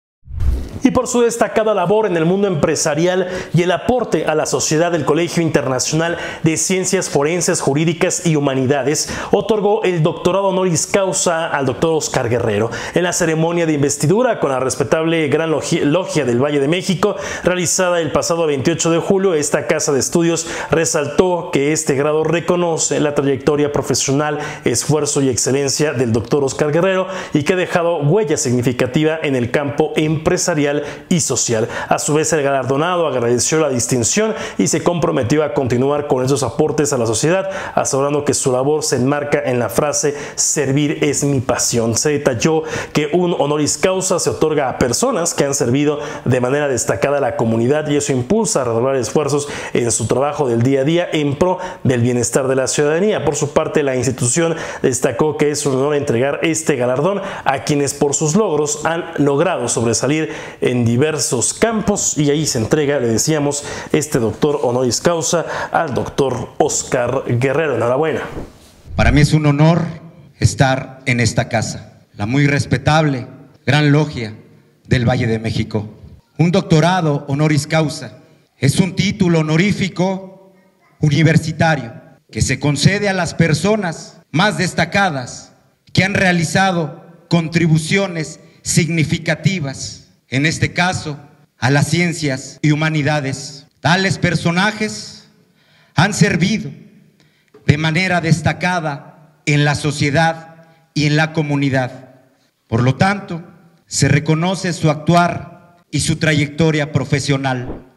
Óscar Guerrero recibe doctorado honoris causa
Por su destacada labor en el mundo empresarial y aporte a la sociedad  el Colegio Internacional de Ciencias Forenses, Jurídicas y Humanidades otorgó el Doctorado Honoris Causa a Oscar Guerrero.

En la ceremonia de investidura con la respetable “Gran Logia del Valle de México ", realizada el pasado 28 de julio, esta casa de estudios resaltó que este grado reconoce la trayectoria profesional, esfuerzo y excelencia del doctor Oscar Guerrero y que ha dejado una huella significativa en el campo empresarial y social.

A su vez el galardonado agradeció la distinción y se comprometió a continuar con estos aportes a la sociedad porque su labor se enmarca en la frase “Servir es mi pasión “.

Detalló que un honoris causa se otorga a personas que han servido de manera destacada a la comunidad y eso lo impulsa a redoblar esfuerzos en su trabajo del día a día en pro del bienestar de la ciudadanía.

Por su parte, la institución destacó que es un honor entregar este galardón a quienes por sus logros han logrado sobresalir  en diversos campos.